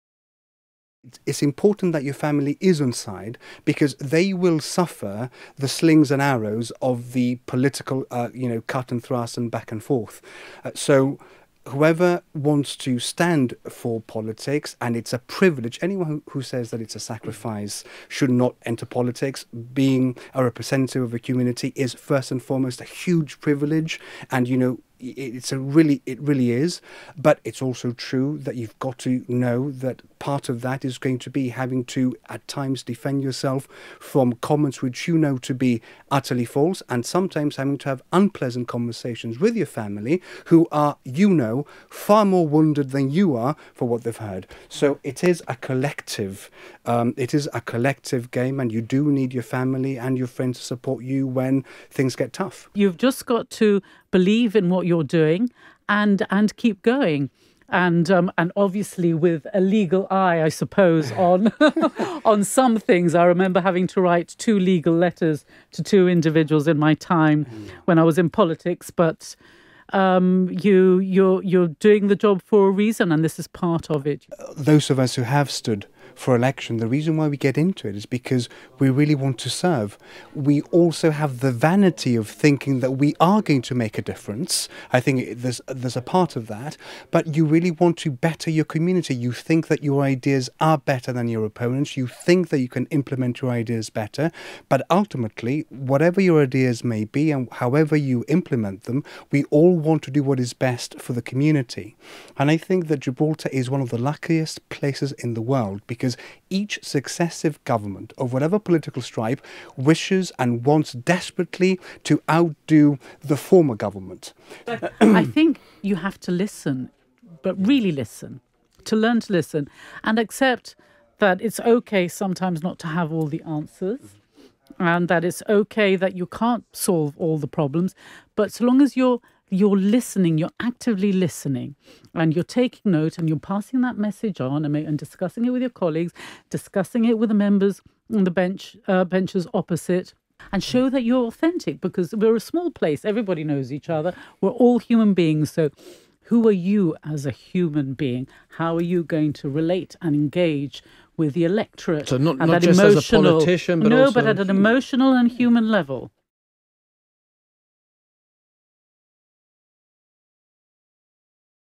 What goes through the mind of someone thinking of becoming an election candidate?
What conversations do they need to have?

We asked former members of parliament who will NOT be putting themselves forward this time round.